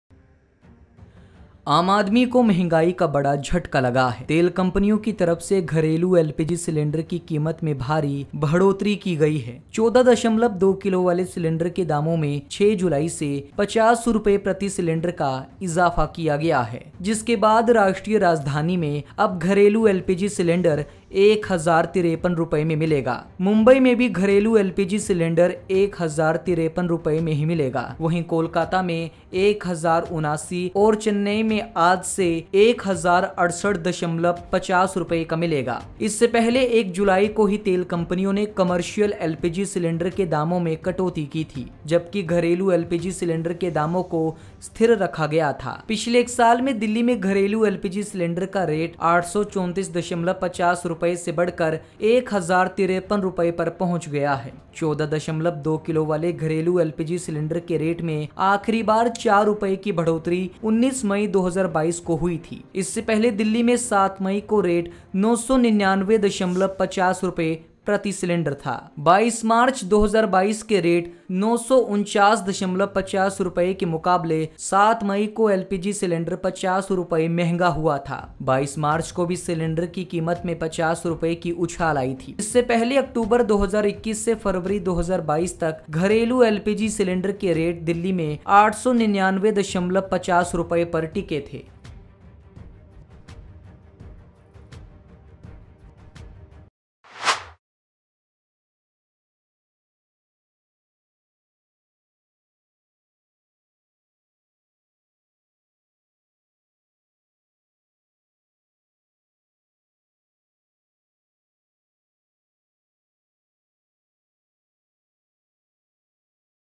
LPG Price Hike : आम आदमी को महंगाई का बड़ा झटका, 50 रुपए महंगा हुआ घरेलू LPG सिलेंडर
आम आदमी को महंगाई (Inflation) का बड़ा झटका लगा है। तेल कंपन‍ियों की तरफ से घरेलू एलपीजी स‍िलेंडर (Domestic LPG Cylinder) की कीमत में भारी बढ़ोतरी की गई है। 14.2 किलो वाले सिलेंडर के दामों में 6 जुलाई से 50 रुपये प्रति सिलेंडर का इजाफा किया गया है। जिसके बाद राष्ट्रीय राजधानी में अब घरेलू LPG सिलेंडर 1053 रुपये में मिलेगा।
    The common man has suffered a major setback due to inflation. There has been a huge increase in the price of domestic LPG cylinder by the oil companies. The price of 14.2 kg cylinder has been increased by Rs 50 per cylinder from July 6. After which domestic LPG cylinder will now be available in the national capital for Rs 1053.